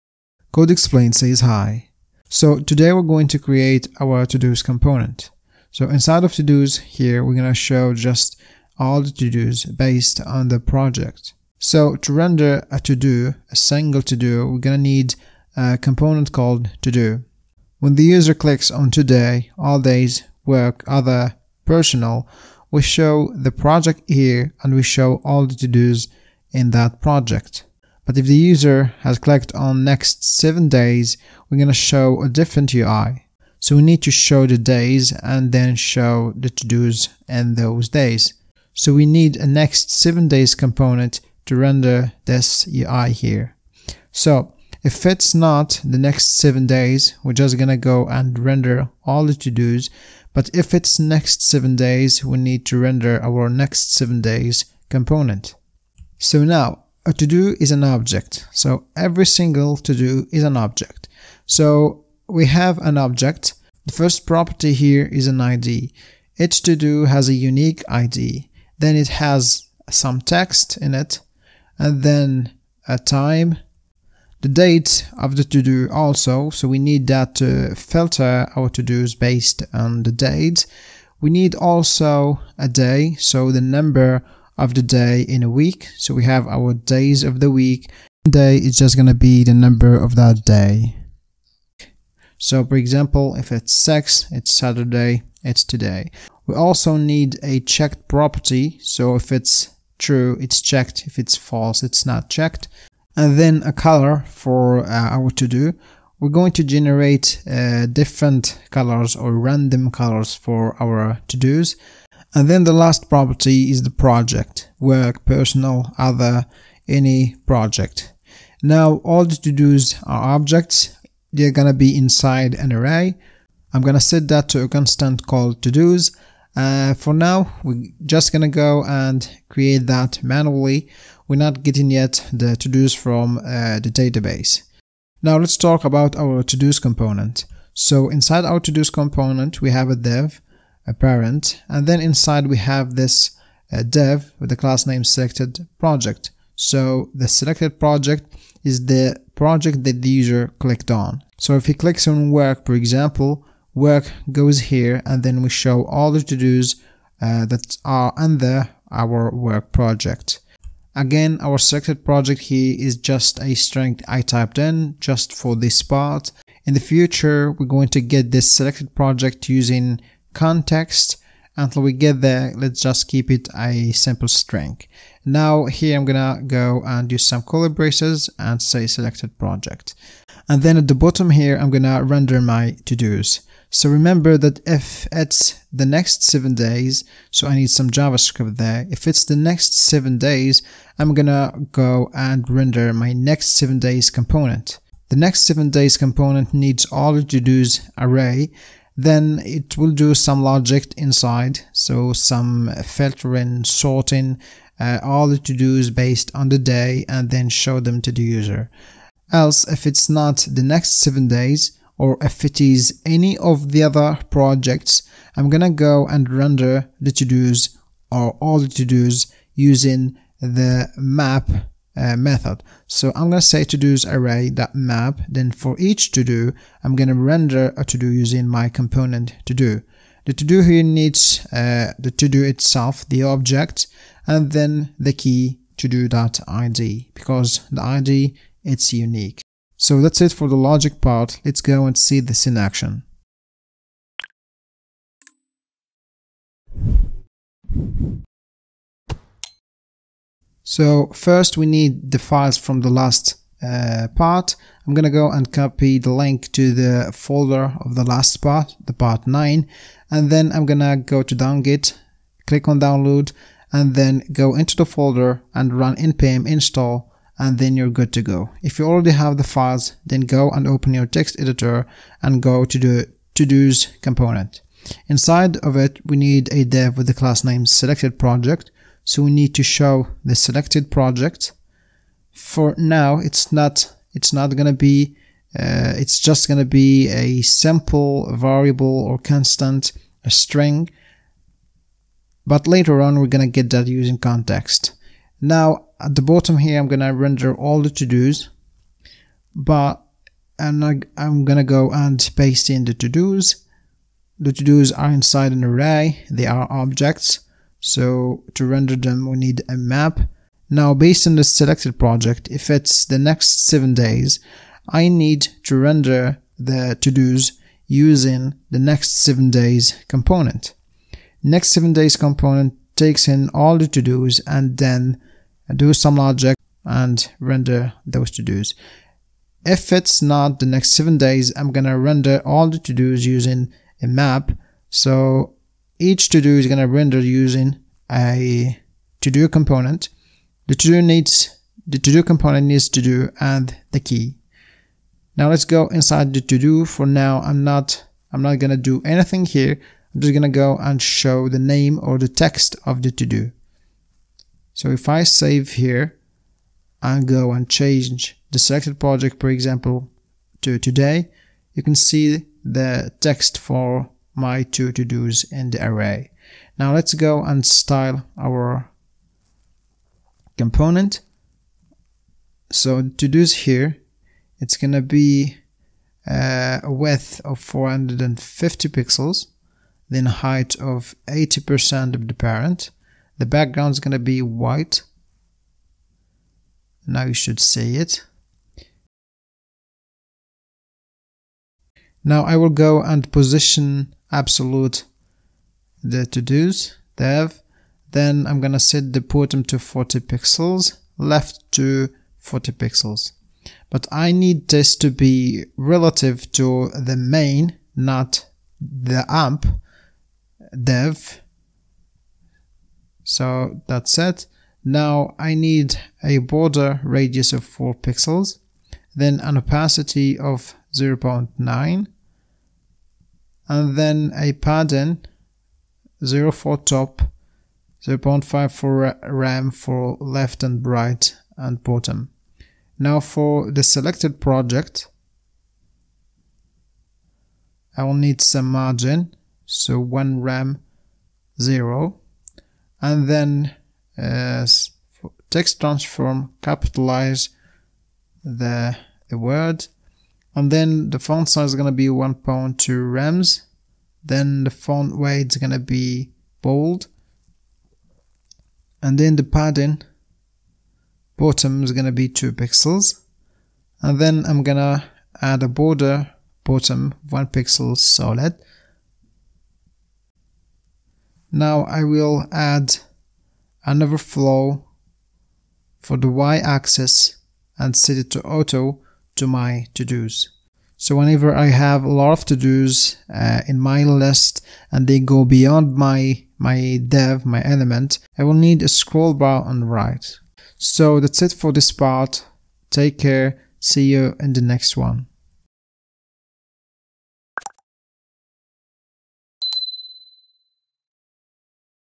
Build a TODO app with React and Firebase • Todos Component • PART 10
PART 10 DESCRIPTION

‎@Code Explained GitHub Repo LINK

COURSE DESCRIPTION

In this course I will walk you through all the steps needed from start to finish to code a To Do App using React JS and firebase. I will explain the logic behind each line of code before typing the code, I'm going to use diagrams and animations so you can follow up easily and have a better understanding of what's going on behind the scenes of our App/Code.

In our To Do App the user can add a to do by filling a form (Controlled component) that has an input for the to do text, an input for date and time (We wi use the date and time pickers from Material UI), and he/she can select the project he/she wants to add the to-do to.

The user can filter the todos by date (Today, Next 7 days, All days) or by project, se when he/she clicks on project, only the to dos under that project will appear.

The user can add, remove or edit a to do. When editing a to do the user won't need to hit a "save button" to save the changes cause the changes ei be saved automatically.

The user can add, remove, rename a project. When removing a project, all the todos under that project will be removed.

When the user completes a to do, we show a line through the text of the to do and show a Circle Check Icon instead of a Circle (We are going to use the icons from react bootstrap icons).

When a to do is completed, the user can repeat the to do next day.

We will add some animations (transitions) to our App, using react spring library.